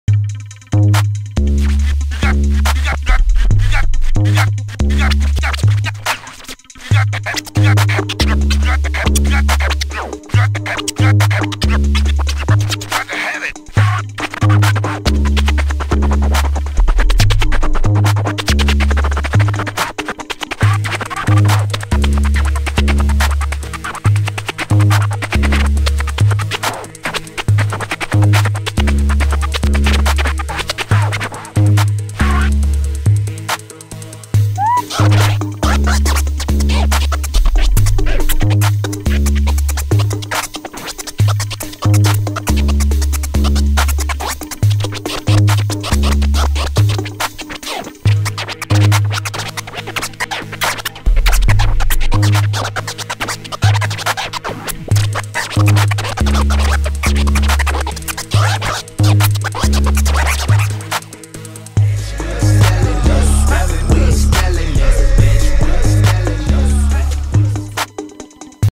DjHypnotize Qbert Skratch University Valedictorian entry 2017
Here is my entry for 2017 scratch battle, big up to kurteek who also is the 2017 IDA USA Champion.  Still scratching and training at QSU developing my sound, come skratch with the best see you next year!     --H